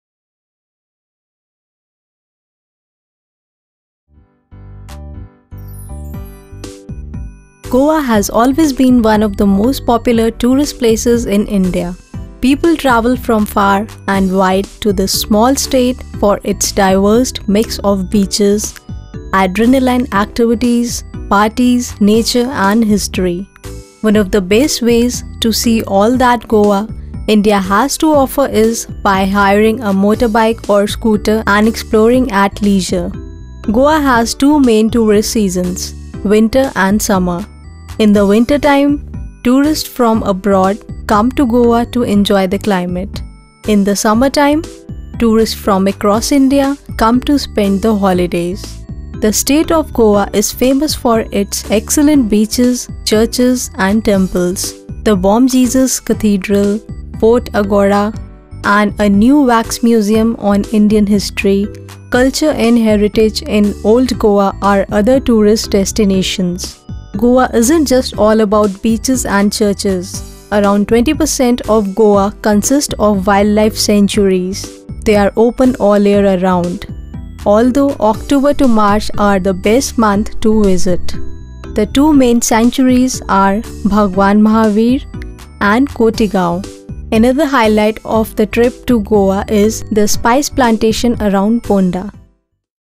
GOA - BEST BEACH VACATION IN INDIA
Trip to GOA
Voice-Akanksha Singh

Goa has always been one of the most popular tourist places in India. People travel from far and wide to this small state for its diverse mix of beaches, adrenaline activities, parties, nature, and history. One of the best ways to see all that Goa India has to offer is by hiring a motorbike or scooter, and exploring at leisure.Goa has two main tourist seasons: winter and summer. In the winter time, tourists from abroad (mainly Europe) come to Goa to enjoy the climate. In the summertime (which, in Goa, is the rainy season), tourists from across India come to spend the holidays.The state of Goa is famous for its excellent beaches, churches, and temples. The Bom Jesus Cathedral, Fort Aguada and a new wax museum on Indian history, culture and heritage in Old Goa are other tourism destinations.
Goa India isn't just all about beaches and churches. Around 20% of Goa consists of wildlife sanctuaries. They're open all year round, although October to March are the best months to visit.The two main sanctuaries are Bhagwan Mahvir (of which Mollem National Park is a part) and Cotigao. Another highlight of a trip to Goa India is the spice plantations around Ponda.